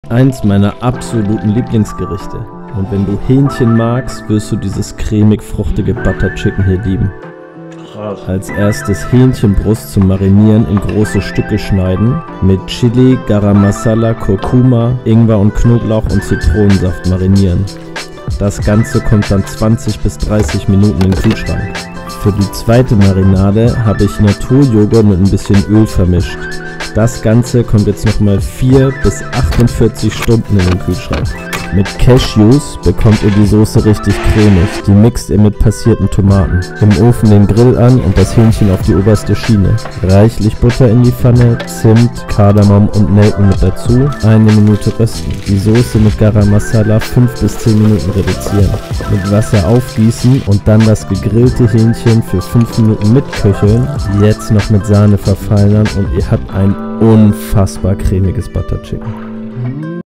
Easy Butter Chicken
Einfach indisch kochen!

Butter Chicken ist hierzulande eines der beliebtesten Currys und gleichzeitig eines der einfachsten! Die Zutaten sind einfach zu beschaffen und erfordern keine Reise zu einem indischen oder asiatischen Supermarkt. Denn du bekommst alles im Supermarkt um die Ecke. Das Fleisch ist butterzart und die Soße cremig und würzig.